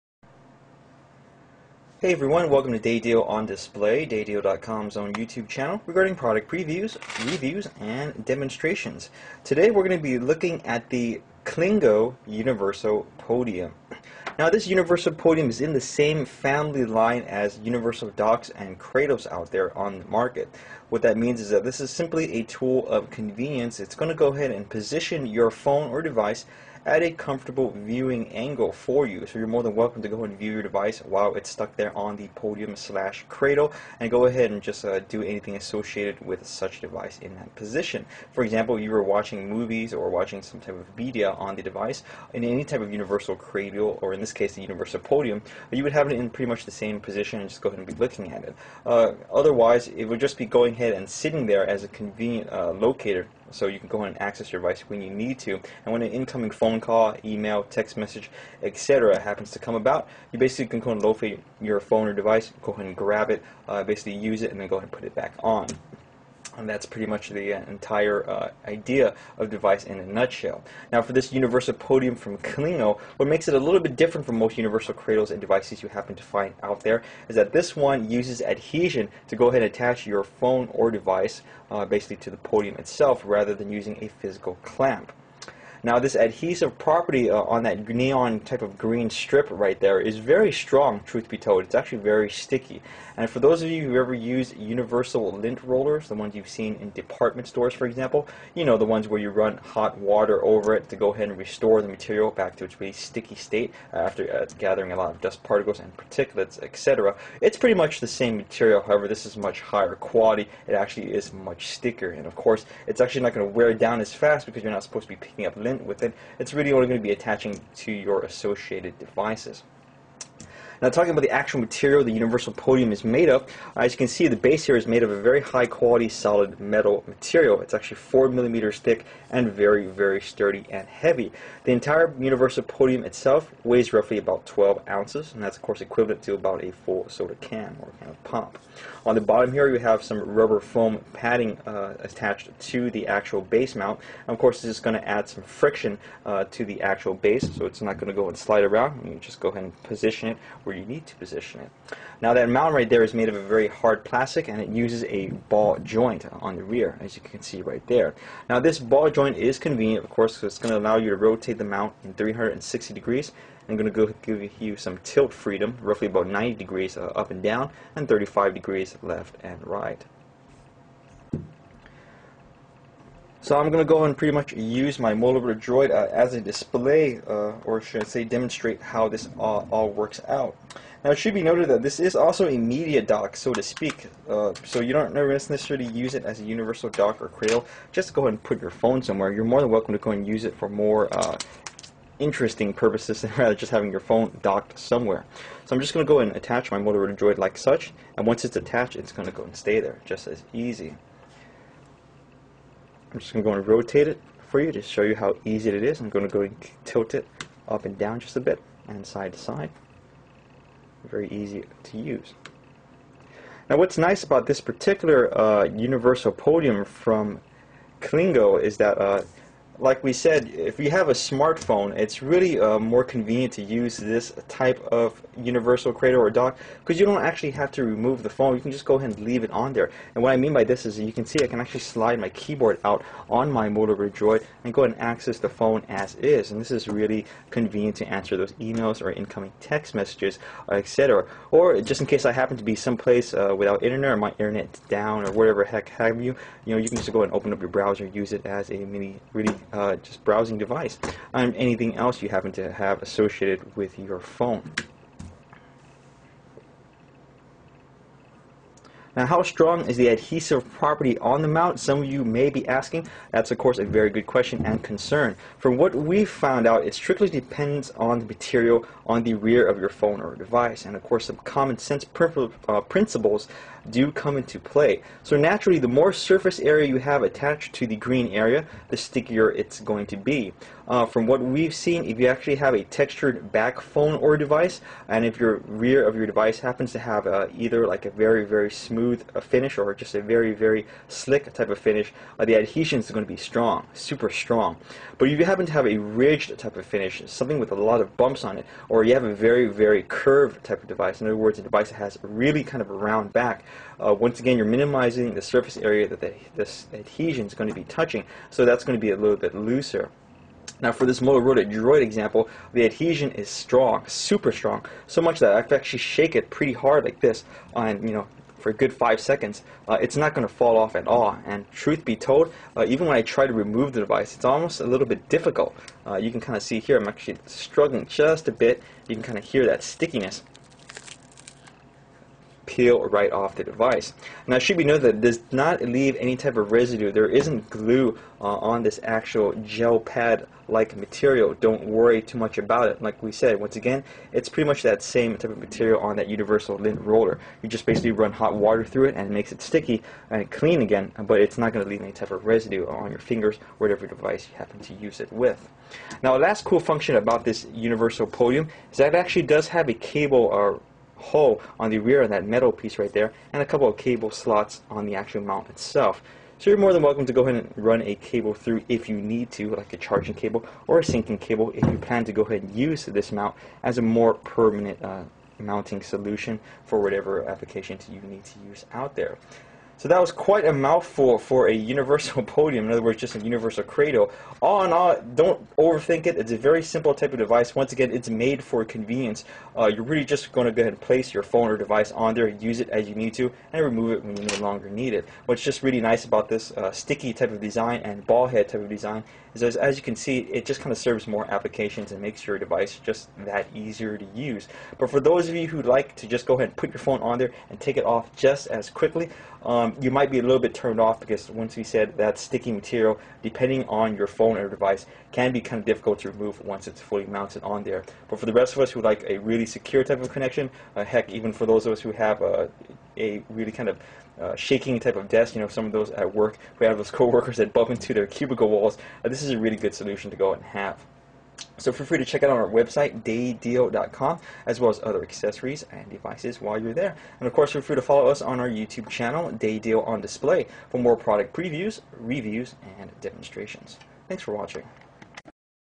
Clingo Universal Podium hands free device holder
The Clingo 07005 Universal Podium Cell Phone Dock is ideal for use in your home or at your office to provide a secure mount for your phone when charging, syncing or accessing your media device - all at the same time. No adapters necessary...ever! You can rest easy knowing you have a safe place to put your phone so you never have to scramble to find it as you are running out the door.

What It Is & Why You Need It:

Multi-axis joint allows for infinite viewing angles.
Solid steel base provides unparalleled stability.
Allows easy, hands-free viewing of calls, text, emails, movies and other apps.
Great for use at home or the office on your desk, kitchen counter, coffee table, nightstand or where ever you happen to use your phone.
Universal - any phone, anywhere.

The Podium's multi-axis joint delivers 360 degree rotation to provide you with infinite viewing angles. This helps to organizes your desk and enables full access to your smartphone features, giving you easy, hands-free viewing of calls, text, emails, movies, and other apps. It even makes it easy for you to check game scores or watch YouTube™ while plugging away at work.

It's pretty simple, really. Clingo holds your phone. It doesn't matter if it is a BlackBerry, iPhone, Droid, Samsung, or whatever. Whether in your car, at the office, at home or wherever - Clingo holds it. Get the Clingo 07005 Universal Podium Cell Phone Dock today